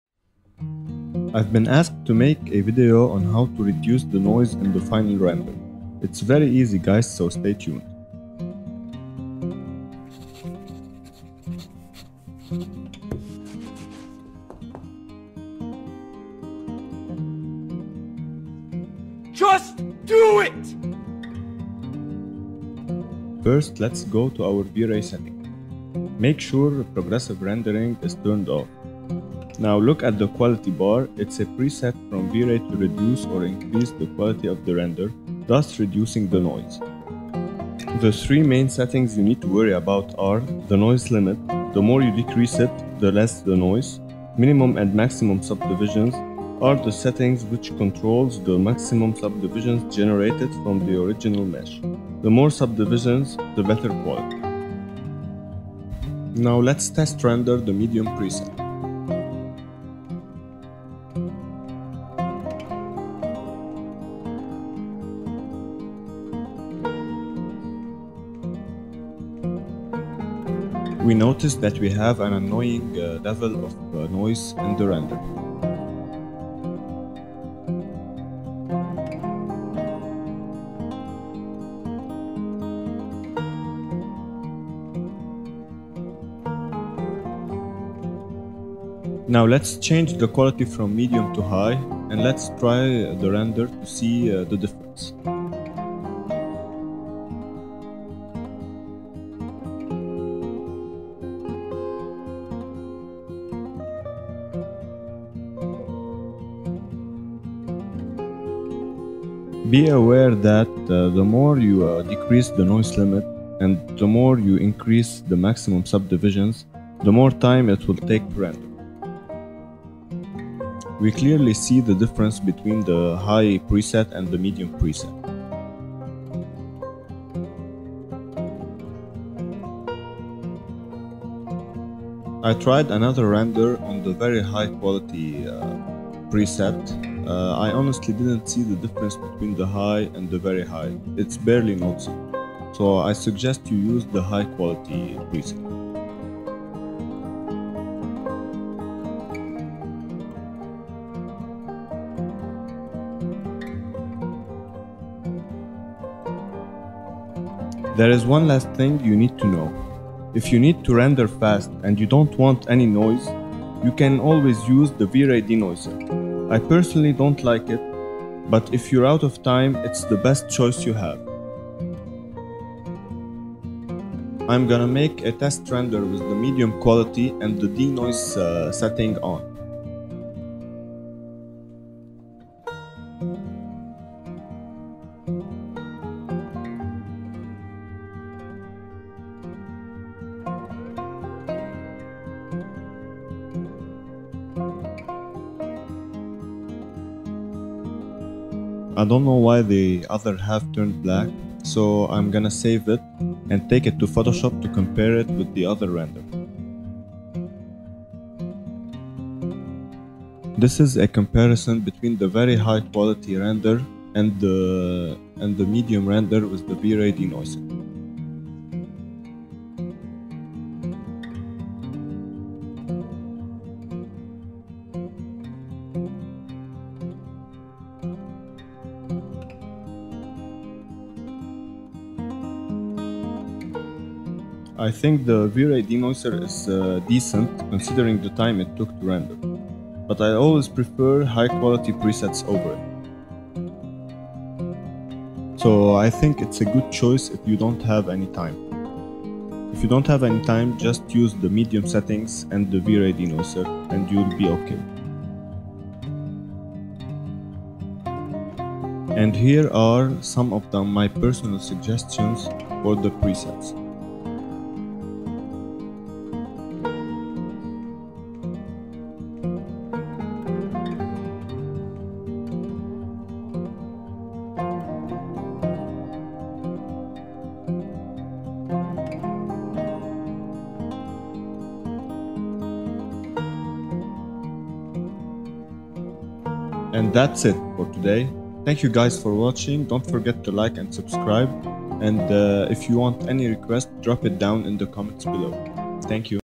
Learn to reduce noise in the render - Vray Next and Sketchup 2019
In this video, I explain how to reduce the noise in your render with one easy step. I hope this will be helpful in your renders!

📹VIDEO LICENSE 👉🏽 Videoblocks